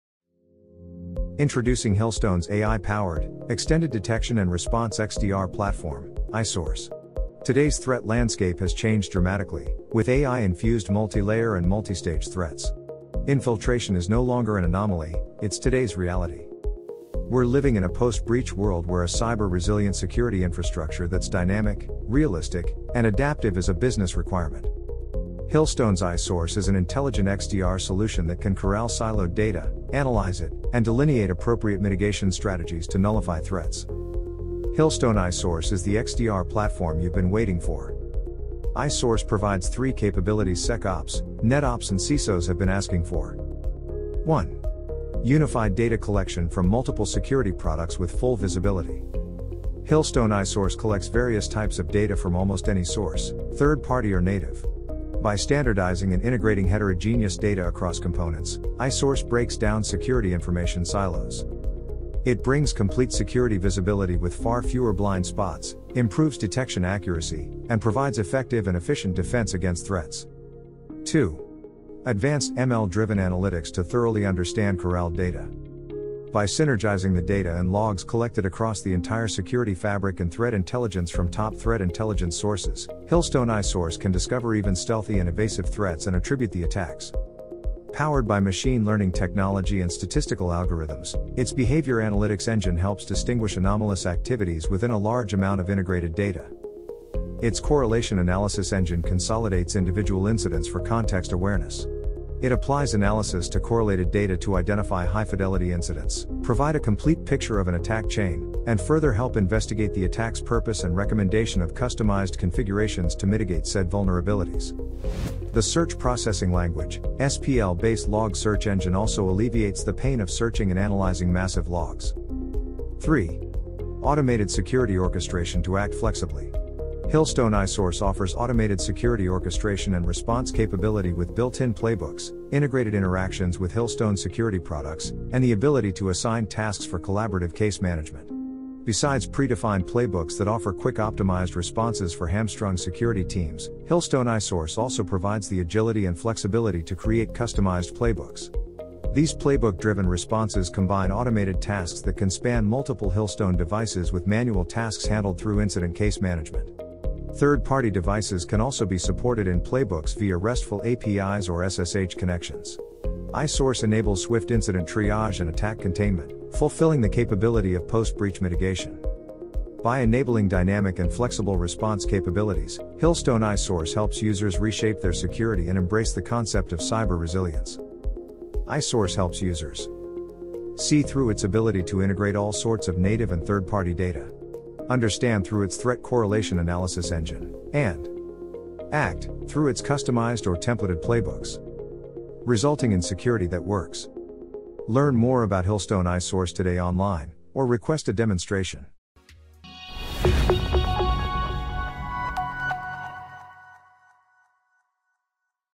Hillstone XDR Solution
Hillstone iSource is a data-driven, AI-powered XDR platform that integrates massive security data, correlates and investigates incidents, identifies potential threats, and automatically orchestrates security to respond cohesively across multiple security products and platforms. iSource brings a radical new approach to cybersecurity with unrivaled security operation efficiency.